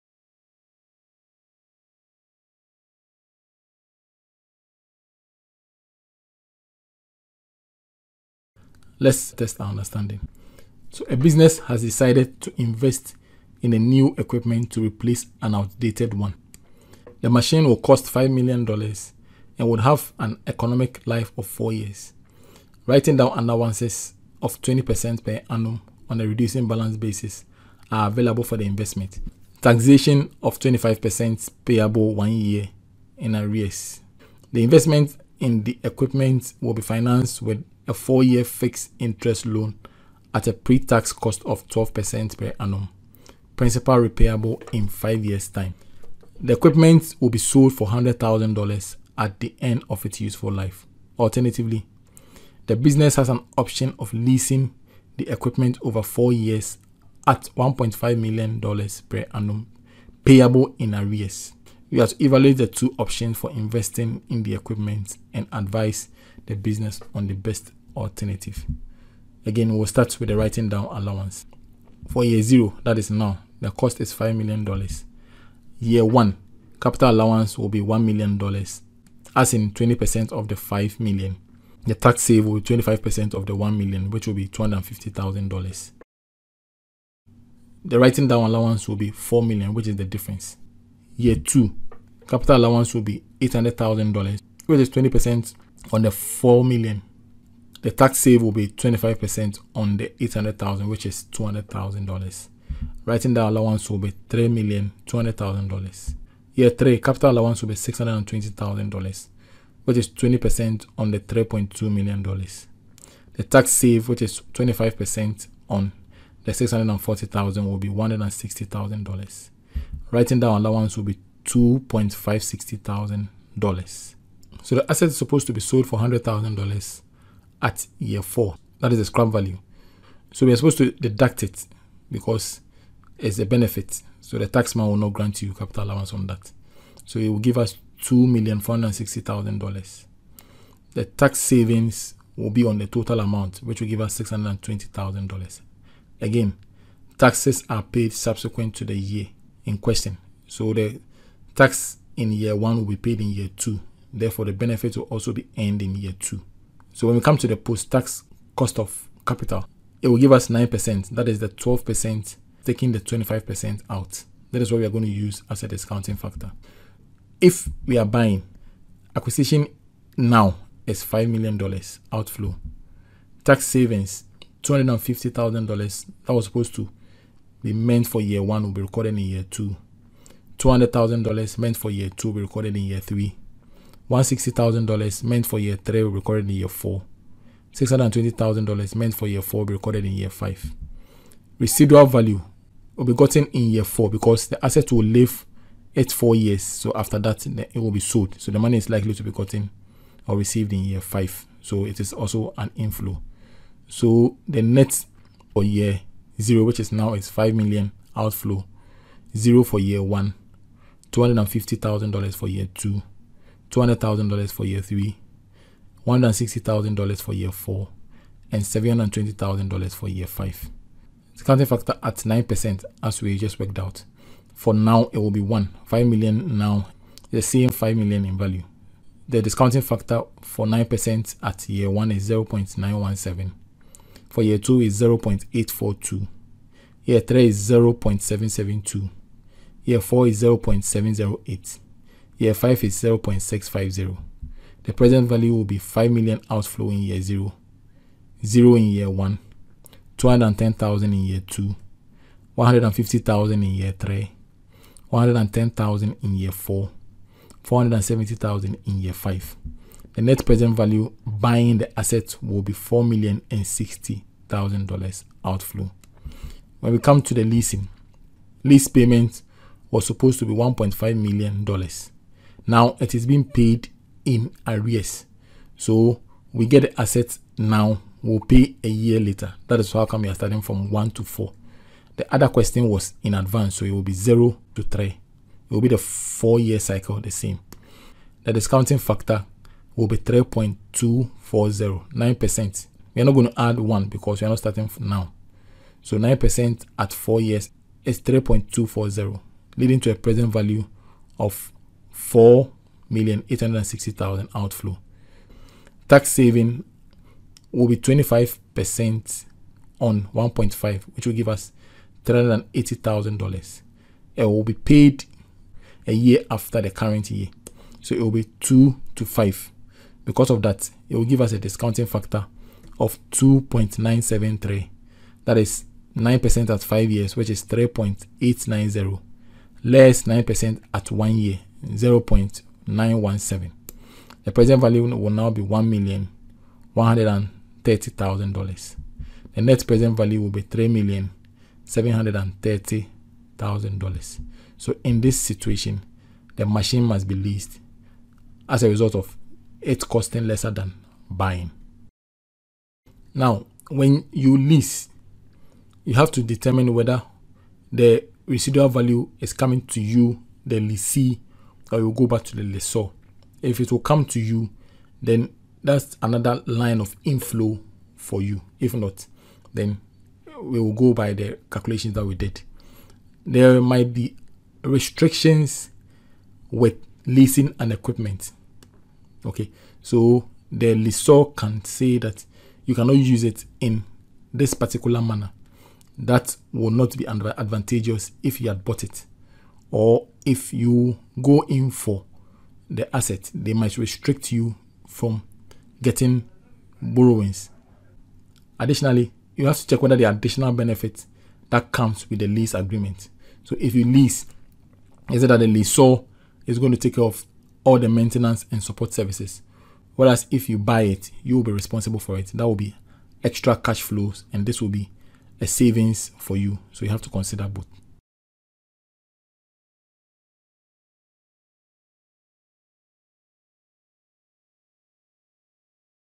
PRACTICAL ILLUSTRATION - Lease or Buy, Investment Appraisal.
A step-by-step practical demonstration of the Lease or Buy system.